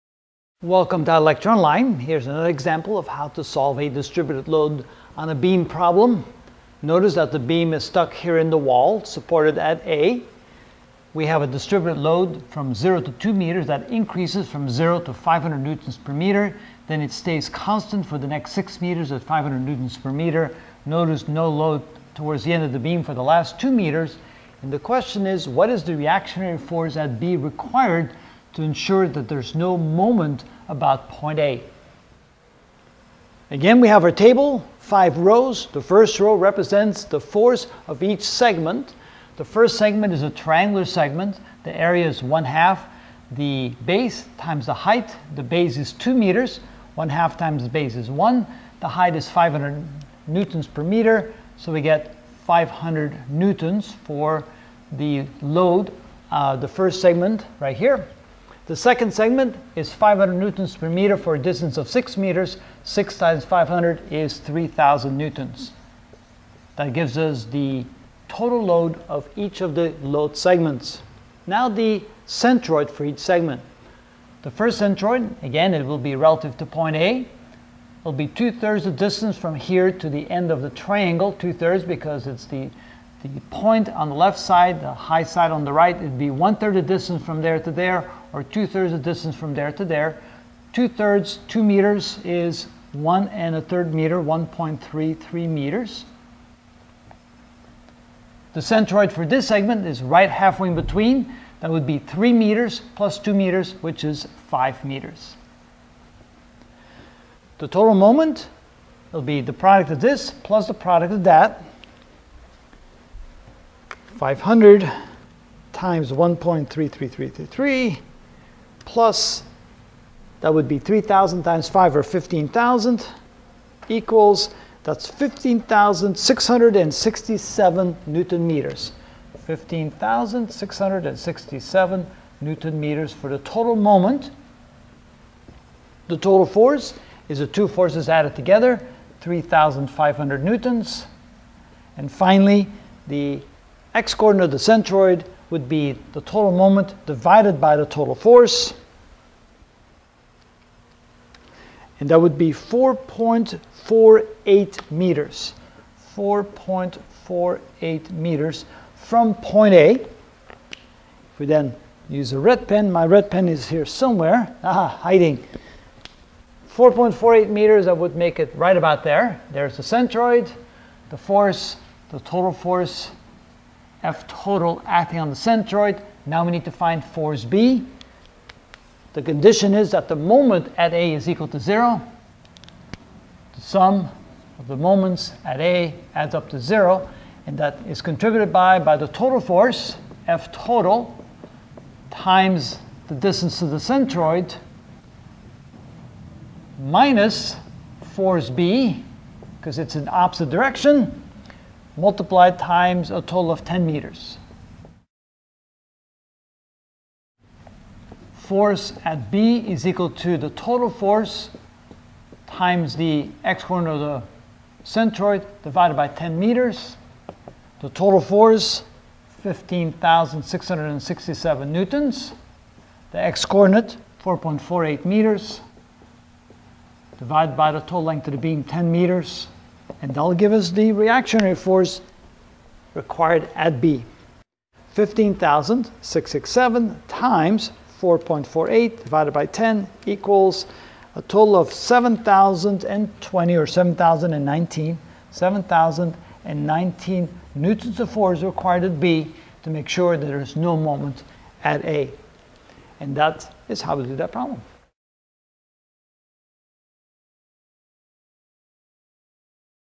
Mechanical Engineering: Distributed Loads on Beams (8 of 12) Find Distributed Load on Beam Ex. 7
In this video I will find (using the table method) the Force at A, and Force total at the centroid of a distributed load on a beam attached at A.